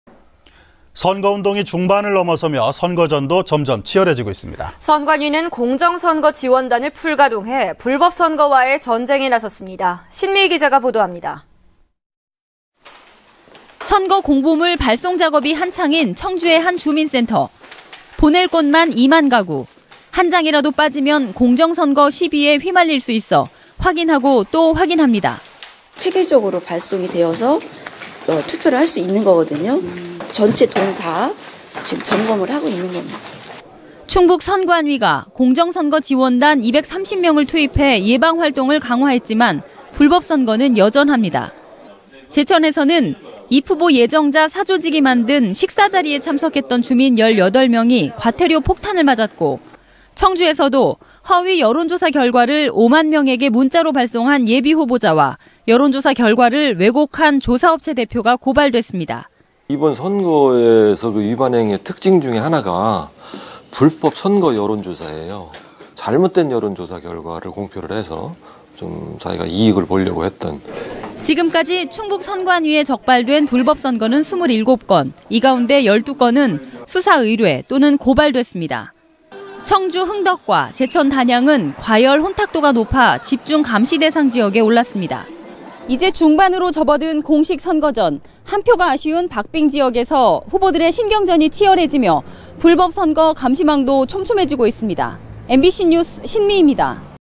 [청주MBC뉴스] 불법선거 감시활동 강화
◀앵커▶
 공식선거운동이 중반을 넘어서며
치열해지고 있습니다.
 선관위는 공정선거 지원단을 풀 가동해,
불법 선거와의 전쟁에 나섰습니다.
신미이 기자가 보도합니다.
           ◀ＥＮＤ▶

 선거공보물 발송작업이 한창인
청주의 한 주민센터.

 보낼 곳만 2만 가구.

 한장이라고 빠지면 공정선거 시비에
휘말릴 수 있어, 확인하고 또 확인합니다.

           ◀ＩＮＴ▶
      송미애/공정선거감시단
   "다 점검하고 있는 거예요."

 충북선관위가 공정선거 지원단 230명을
투입해 예방 활동을 강화했지만,
불법선거는 여전합니다.

 제천에서는 입후보 예정자 사조직이 만든
식사 자리에 참석했던 주민 18명이
과태료 폭탄을 맞았고,

 청주에서도 허위 여론조사 결과를 5만 명에게 문자로 발송한 예비후보자와, 여론조사 결과를 왜곡한 조사업체 대표가 고발됐습니다.

            ◀ＩＮＴ▶
  안홍수/ 충북선관위 홍보담당관

 지금까지 충북선관위에 적발된 불법선거는
27건, 이 가운데 12건은 수사의뢰 또는
고발됐습니다.

 청주 흥덕과 제천단양은 과열 혼탁도가
높아 집중 감시 대상지역에 올랐습니다

 S/U" 이제 중반전으로 접어든 공식 선거전,
한표가 아쉬운 박빙지역에서 후보들의 신경전이 치열해지고 있어, 선관위의 감시망도 촘촘해
지고  있습니다.
MBC NEWS 신미입니다.